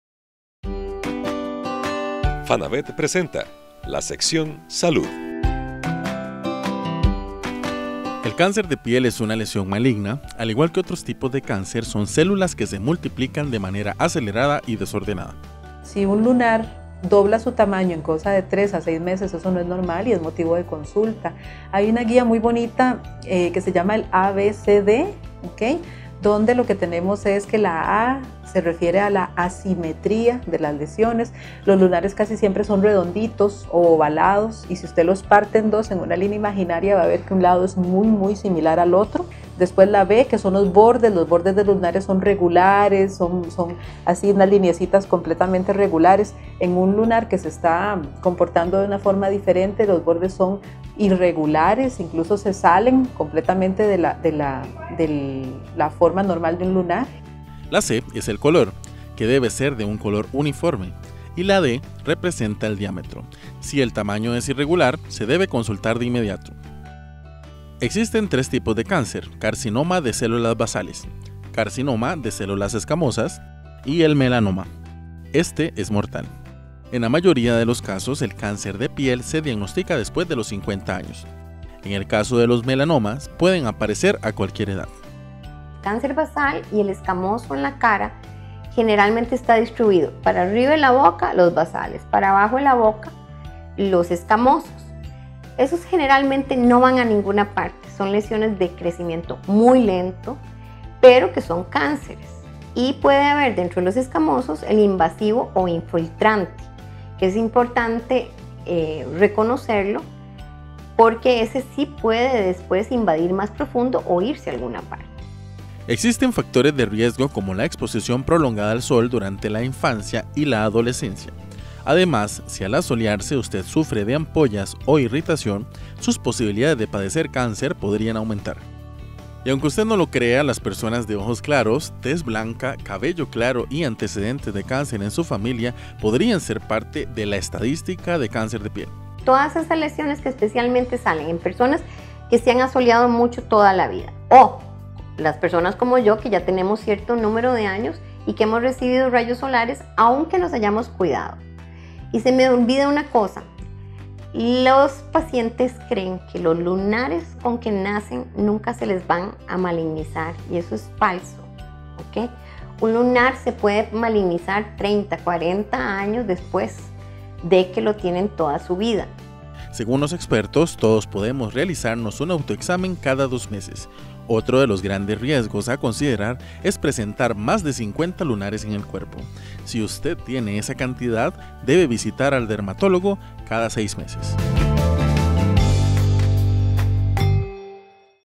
TIPOS DE CÁNCER DE PIEL
Tenga mucho cuidado con el cáncer de piel, le contamos acerca de los tres tipos: